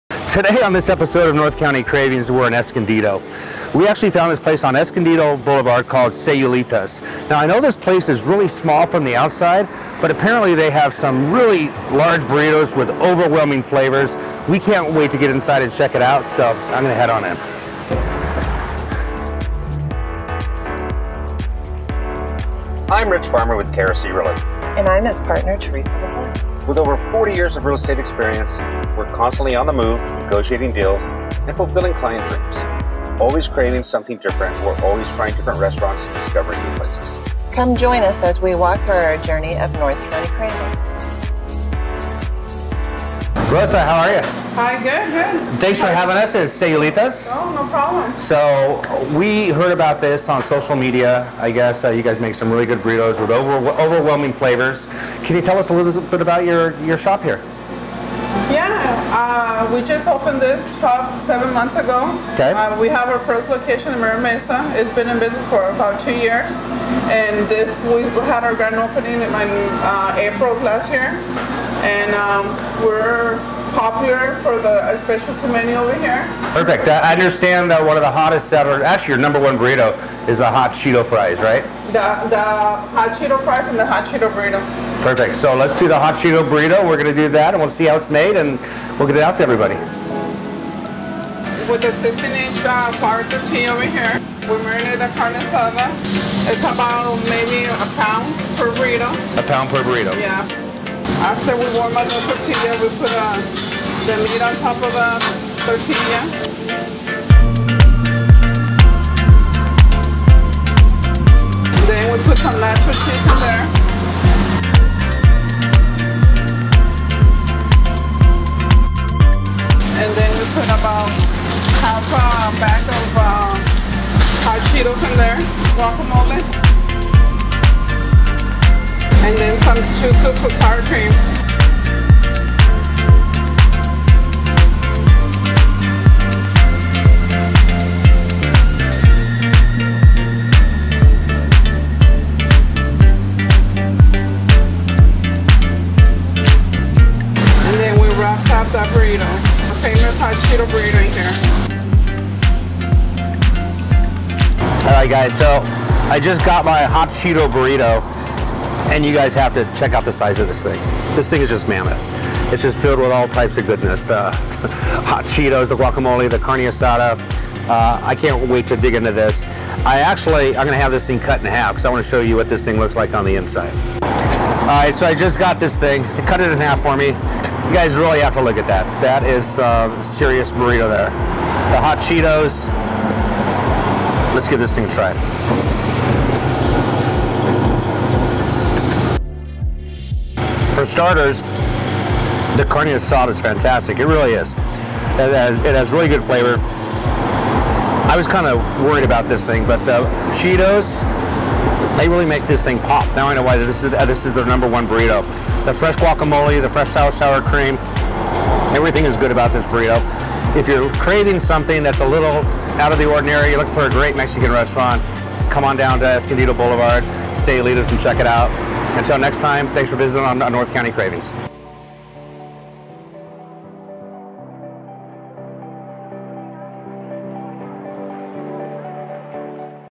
Episode 06 of North County Cravings at Sayulitas in Escondido
Introducing the jumbo 1.5 pound Hot Cheetos Burrito only at Sayulitas, we were so curious of what it has inside and what's the mix taste like, here is our experience trying this burrito for the first time.